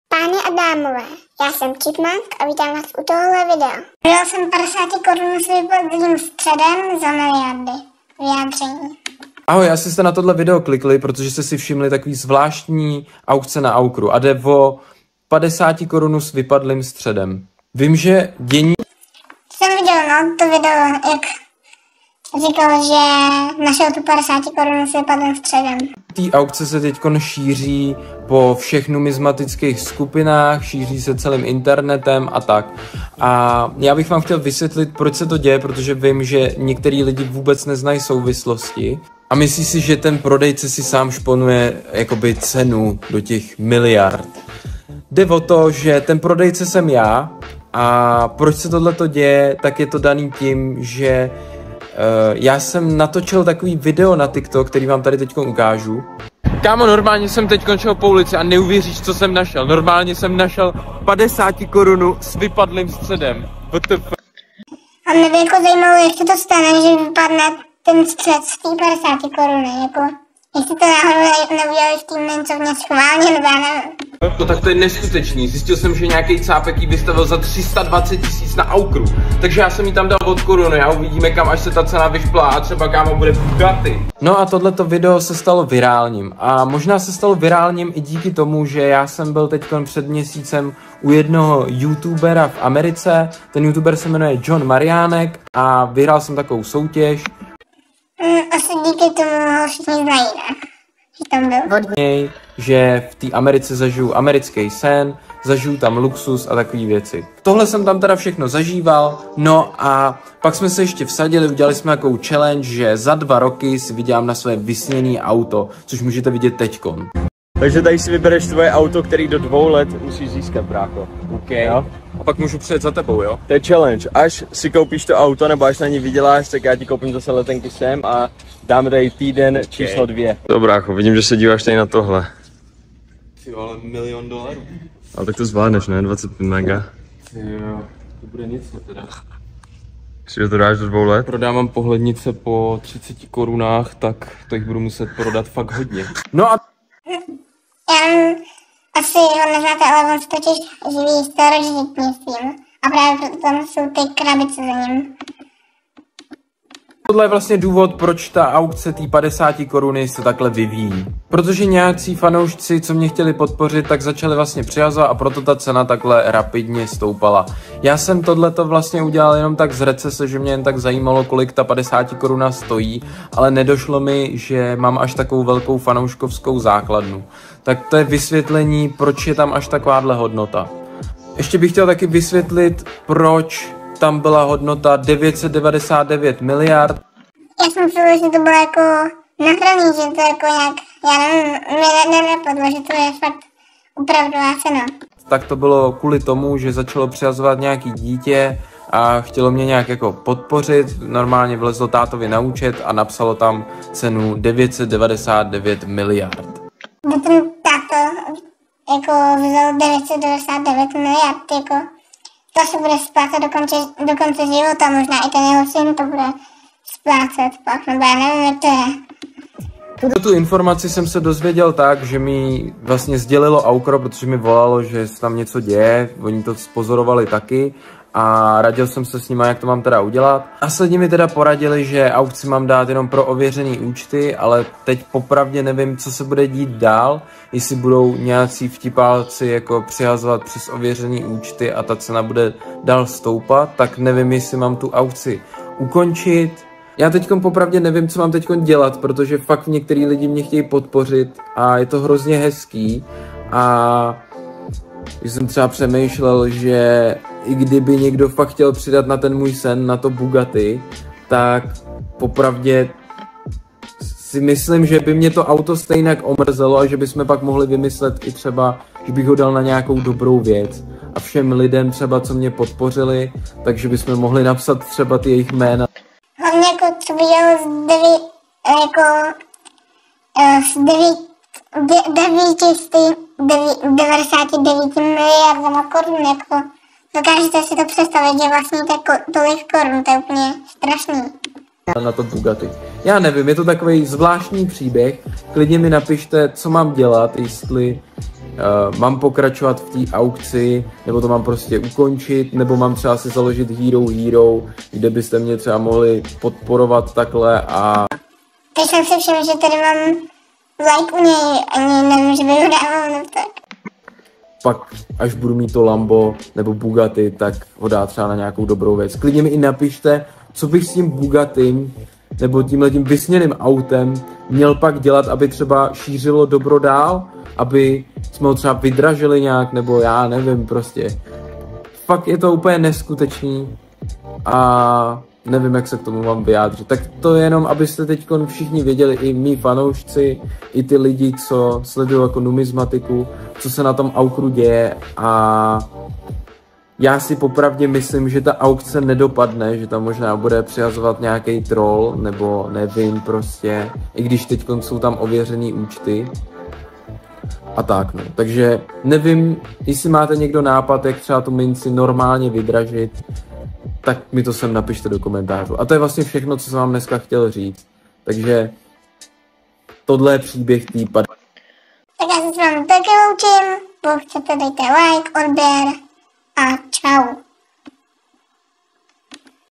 Dítě ukradlo tátovi kreditku a přihodilo 999 miliard korun!
Znáte Jona Marianka? A viděli jste sérii jeduzajonem? A pamatujete si toho divnýho týpka co vyhrál tu soutěž? Tak ten nedávno našel padesátikorunu s vypadlým středem. Dal ji na Aukro a nějaké dítě tam přihodilo 999 miliard korun! Stevenovo vyjádření a další věci zjistíte v tomto videu.